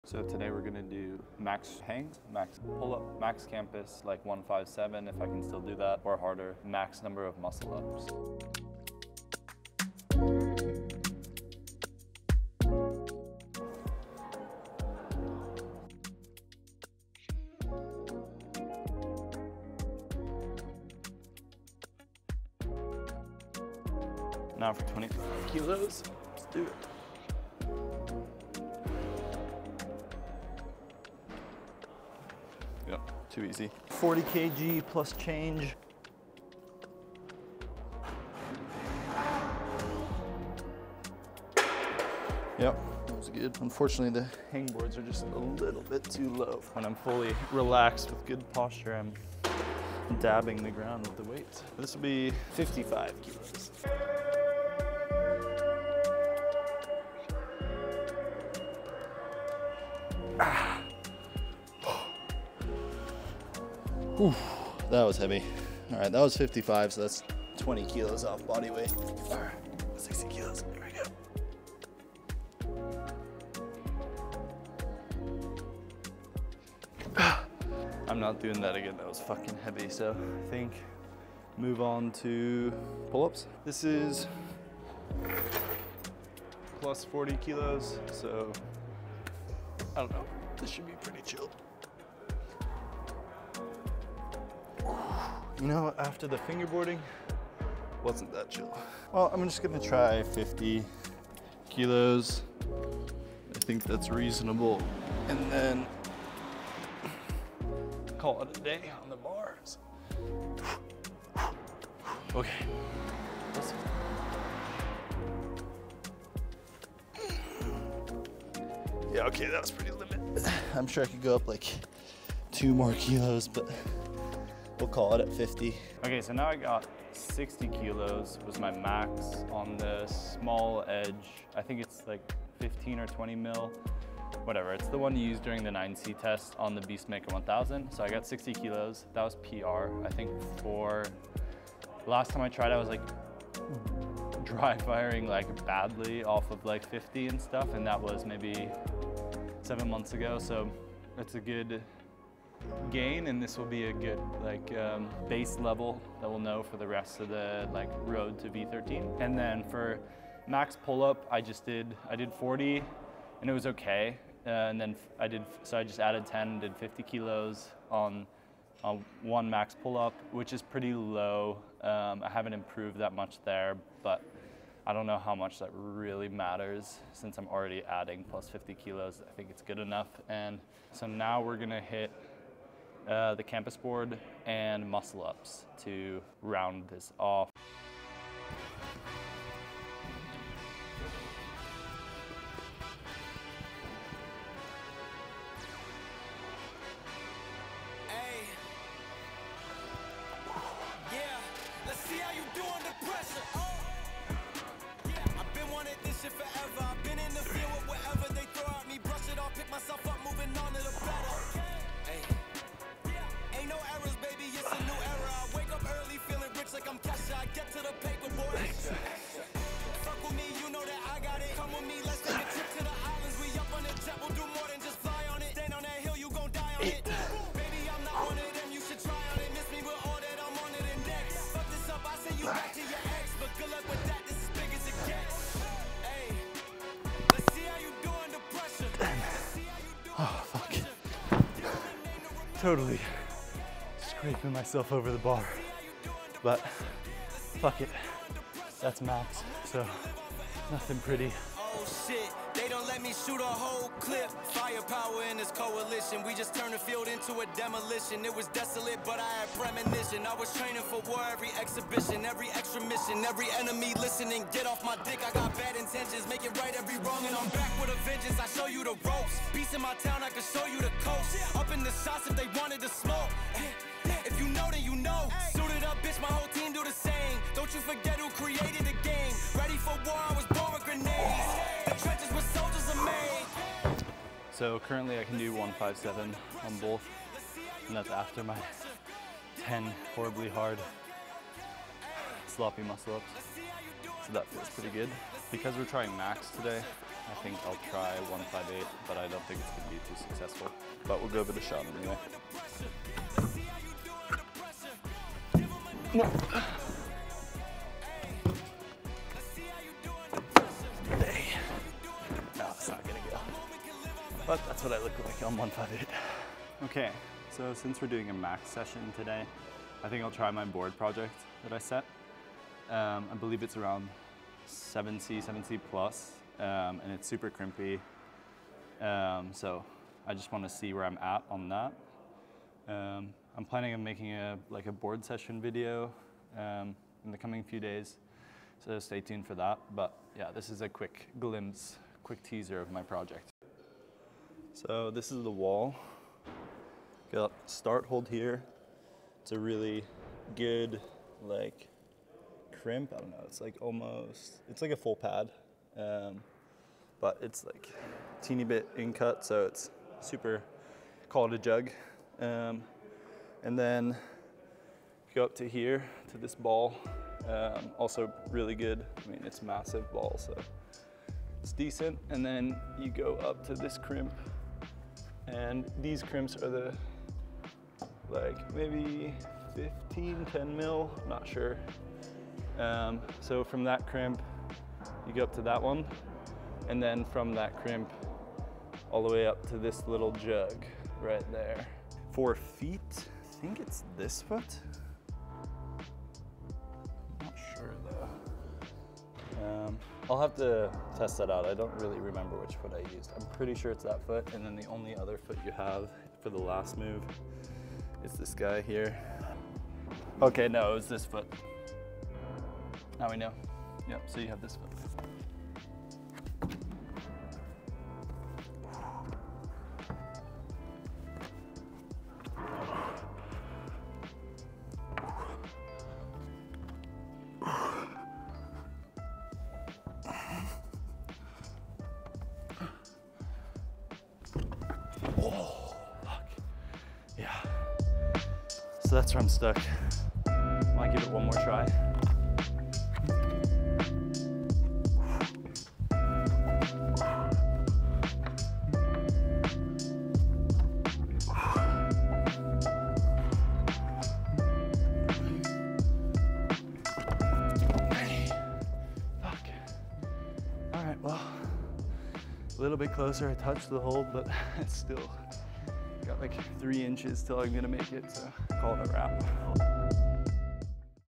I'M NOT STRONG ENOUGH FOR V13
Road To V13: Episode Two

Hard benchmarking session.